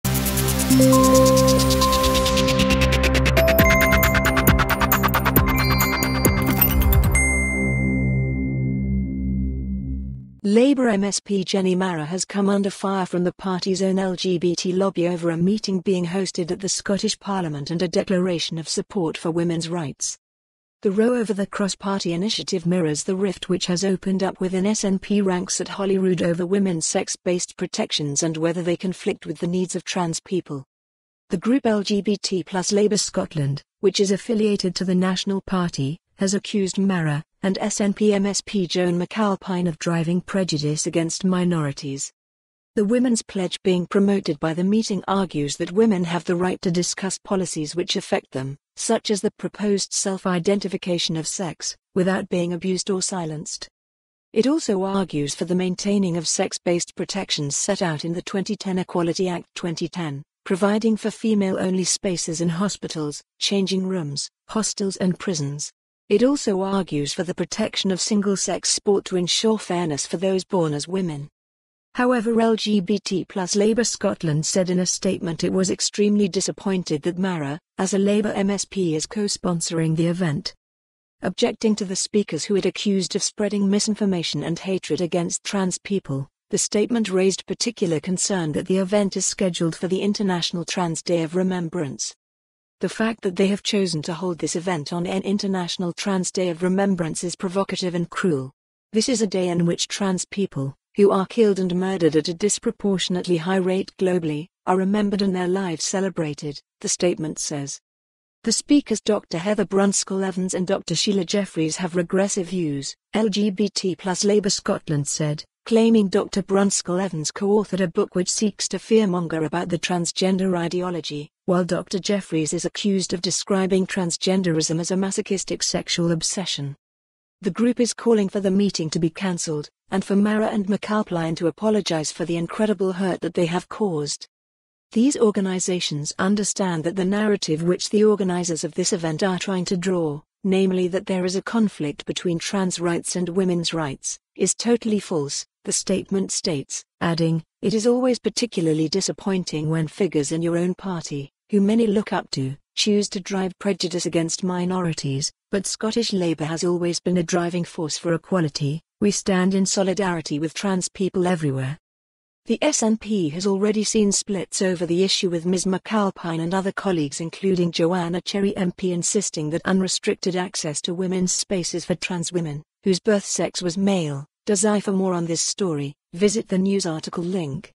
Holyrood rift over womens and trans rights spreads to Scottish Labour
Labour MSP Jenny Marra has come under fire from the party's own LGBT lobby over a meeting being hosted at the Scottish Parliament and a declaration of support for women's rights.

The row over the cross-party initiative mirrors the rift which has opened up within SNP ranks at Holyrood over women's sex-based protections and whether they conflict with the needs of trans people.

The Women's Pledge being promoted by the meeting argues that women have the right to discuss policies which affect them, such as the proposed self identification of sex, without being abused or silenced.

It also argues for the maintaining of sex-based protections set out in the 2010 Equality Act 2010, providing for female-only spaces in hospitals, changing rooms, hostels and prisons. It also argues for the protection of single-sex sport to ensure fairness for those born as women.

However LGBT+ Labour Scotland said in a statement it was "extremely disappointed" that Marra, as a Labour MSP, is co-sponsoring the event.

Objecting to the speakers who it accused of spreading "misinformation and hatred against trans people", the statement raised particular concern that the event is scheduled for the international Trans Day of Remembrance.

"The fact that they have chosen to hold this event on n International Trans Day of Remembrance is provocative and cruel. This is a day in which trans people, who are killed and murdered at a disproportionately high rate globally, are remembered and their lives celebrated," the statement says.

The speakers  Dr Heather Brunskell-Evans and Dr Sheila Jeffreys have "regressive" views, LGBT+ Labour Scotland said, claiming Dr Brunskell Evans coauthored a book which "seeks to fear-monger about the 'transgender ideology'", while Dr Jeffreys is accused of describing transgenderism as a "masochistic sexual obsession".

The group is calling for the meeting to be cancelled, and for Marra and McAlpline to apologise for the " incredible hurt that they have caused".

"These organisations understand that the narrative which the organisers of this event are trying to draw, namely that there is a conflict between trans rights and women’s rights, is totally false," the statement states, adding: "It is always particularly disappointing when figures in your own party, who many look up to, choose to drive prejudice against minorities, but Scottish Labour has always been a driving force for equality ... We stand in solidarity with trans people everywhere."

The SNP has already seen splits over the issue with Ms McAlpine and other colleagues including Joanna Cherry MP insisting that unrestricted access to women's spaces for transwomen - whose birth sex was male - does in fact conflict at times with the needs of women, while others, such as MP Mhairi Black condemning such comments as 'transphobic'.

Debate over the issue has become particularly heated online. There was consternation last night after one prominent Edinburgh based trans activist appeared to threaten Marra and McAlpine online.

Cathy Brennan Tweeted: "The reason why joan McAlpine and Jenny Marra can host on (sic) transphobe on trans day of remembrance is because transphobes know there will be no real repercussions.They have no fear. That needs to be fixed".

Asked to clarify, they went further saying: "I'm saying that you should face repercussions for your bigotry and if that isn't possible through official channels then we should take it into their own hands".

Joan McAlpine and Jenny Marra were approached for comment but were not available.